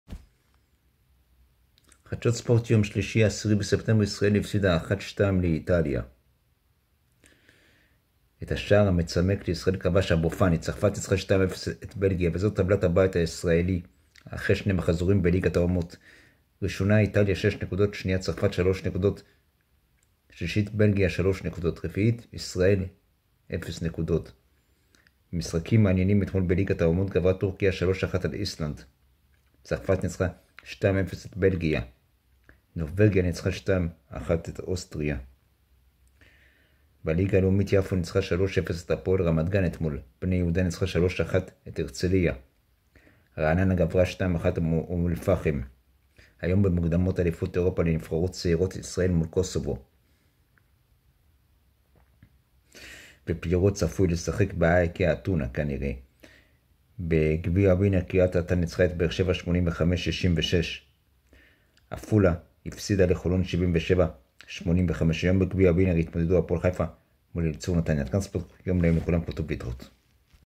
יום 3440 day -חדשות ספורט שלישי 10.09.24
תוספת
קולמוביה 2 ארגנטינה 1
פרגואי 1 ברזיל 0
הולנד 2 גרמניה 2
אנגליה 2 פינלנד 0
ישראל צעירה 0 קוסובו  צעירה 1
הפועל חיפה 81 נתניה 69